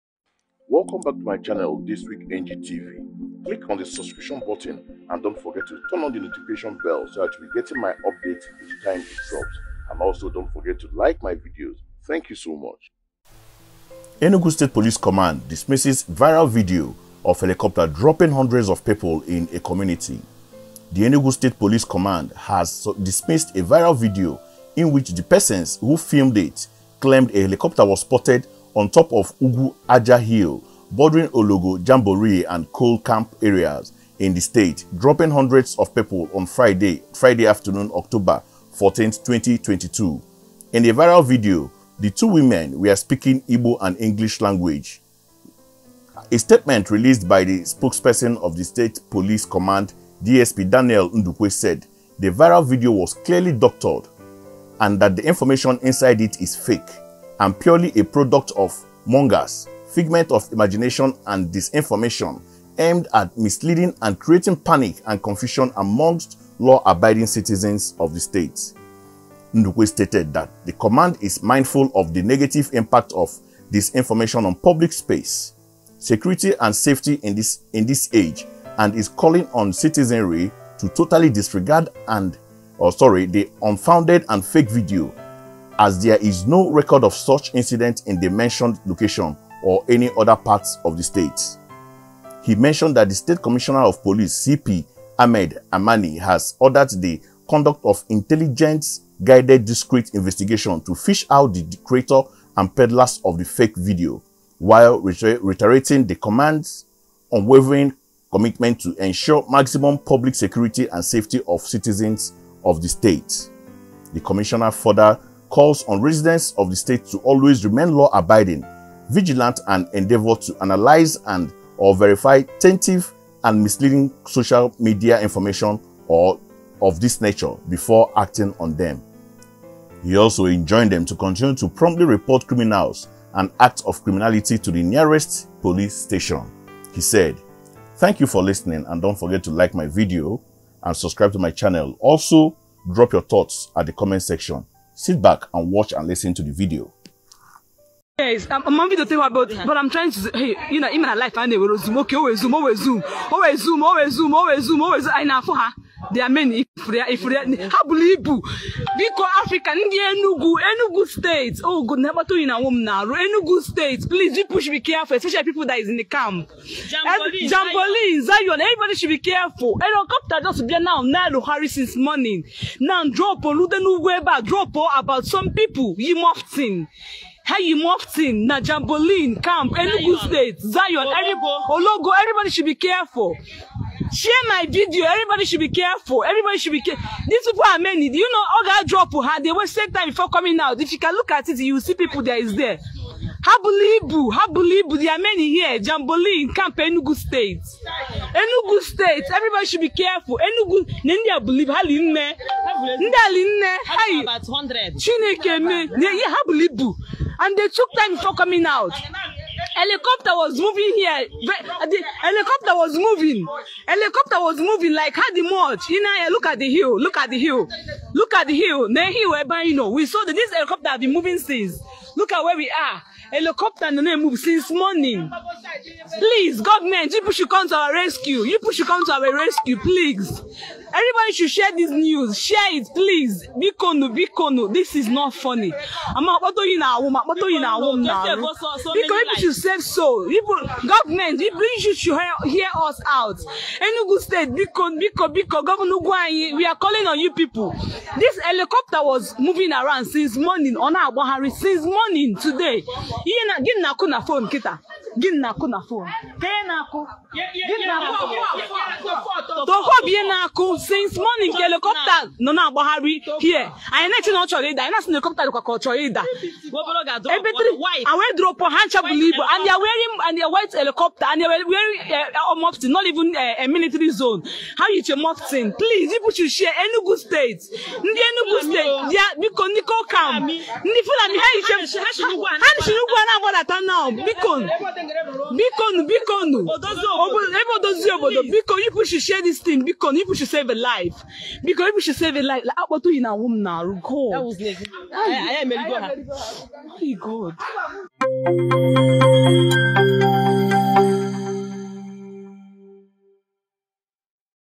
Enugu police command dismisses viral video of helicopter dropping hundreds of people in a community
Fake and disinformative- Enugu state police command dismisses viral video of helicopter dropping hundreds of people in a community

The Enugu state police command has dismissed a viral video in which the persons who filmed it claimed a helicopter was spotted on top of Ugwu-Aja hill bordering Ologo jamboree and Coal camp areas in the state, dropping hundreds of people on Friday afternoon, October 14.

In the viral video, two women speaking Igbo and English language made the claim.

A statement released by the spokesperson of the state police command, DSP Daniel Ndukwe, said the viral video was clearly doctored and that the information inside it is ''fake and purely a product of the mongers' figment of imagination and disinformation, aimed at misleading and creating panic and confusion amongst law-abiding citizens of the State''.